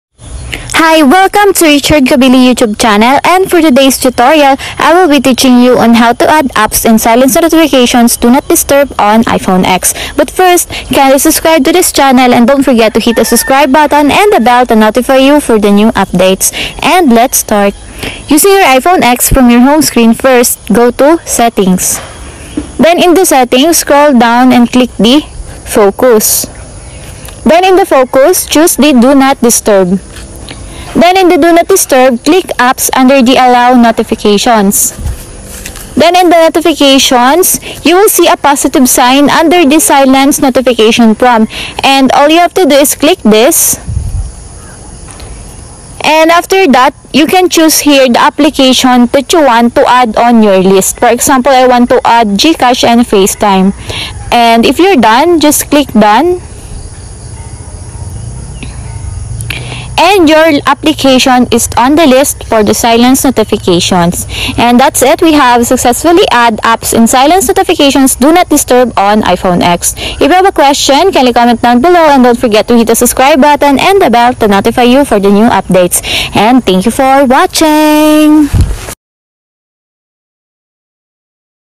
How to add apps in silence notifications do not disturb on iPhone X
Do you want to add apps in silence notifications do not disturb on iPhone X mobile phone.

Thanks...